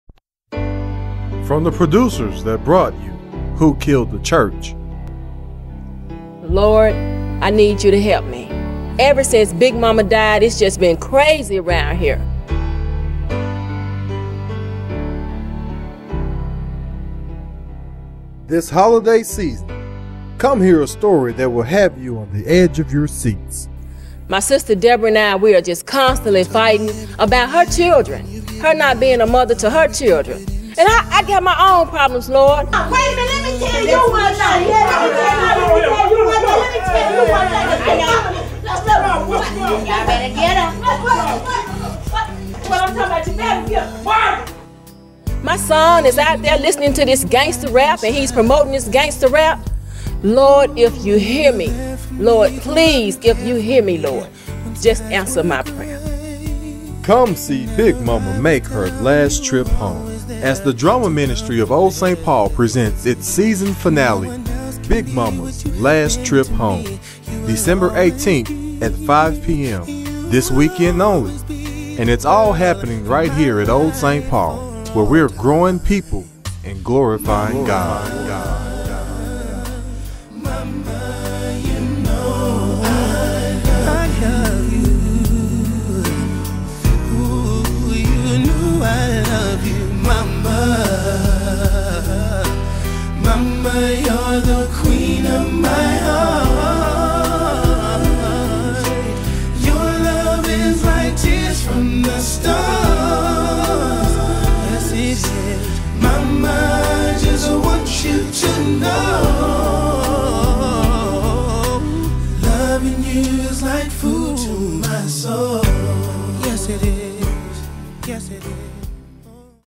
Big Mama's Last Trip Home
Come witness the stage play presented by the Drama Ministry of Old St. Paul as "Big Mama" makes her last trip home...

Saturday, December 18, 2010 beginning at 5pm...

Free Admission!!! Open to the Public!!!
Make Plans to Attend!!!

Old St. Paul M.B. Church
504 S. 8th Street
West Memphis, AR 72301